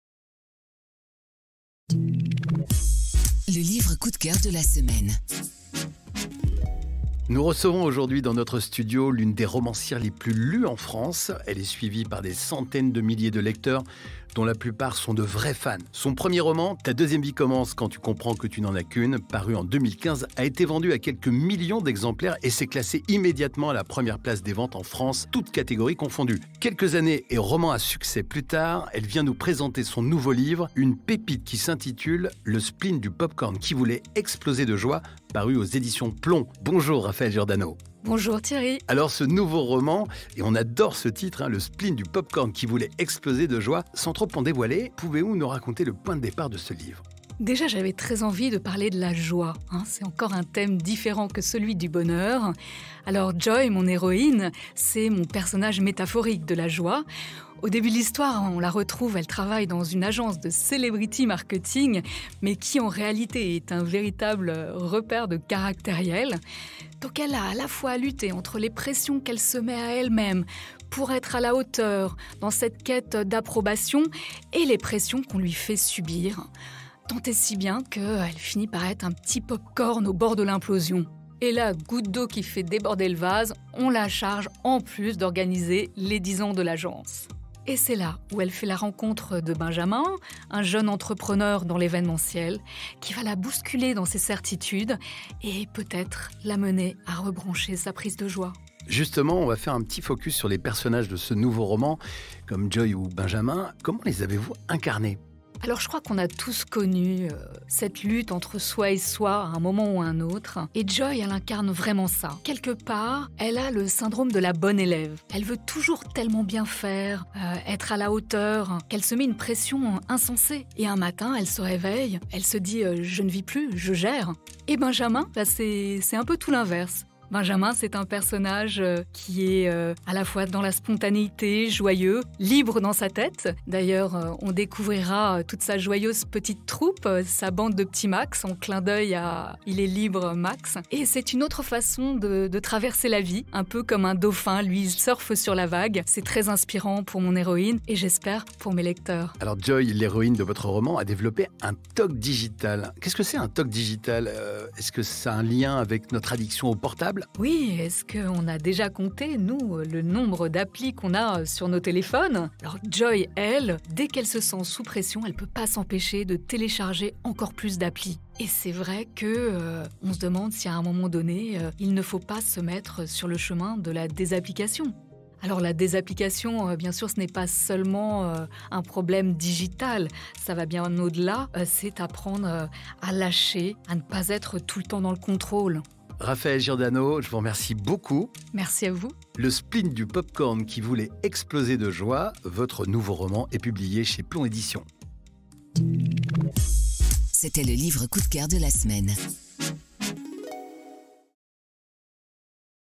[calendrier de l'avent du 14 décembre] - Gagnez votre exemplaire du roman Le Spleen du Popcorn
On t’ouvre notre bibliothèque !
Pour participer :
- 2. Suis le lien pour tenter de remporter l’ouvrage du jour.
 Bonne chance !